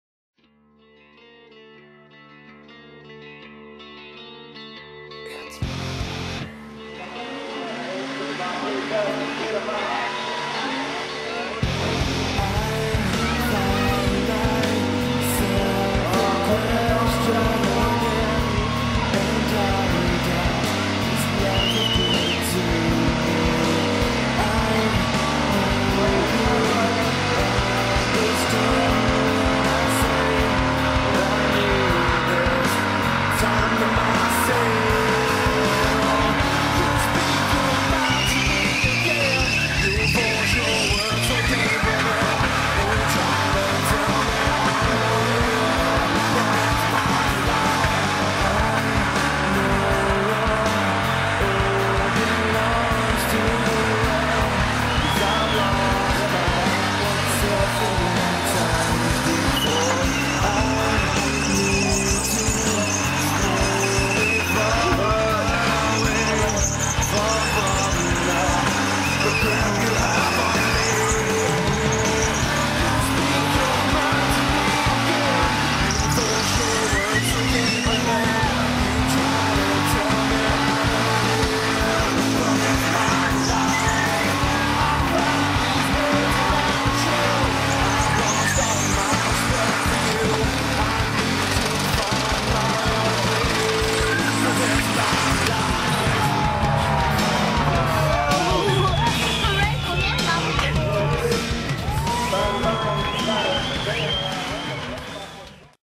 Nick Jones Winner of 6cylinder bracket
Burnout Comp in Charters Towers Round 1 burnout by Nick Jones and his Triton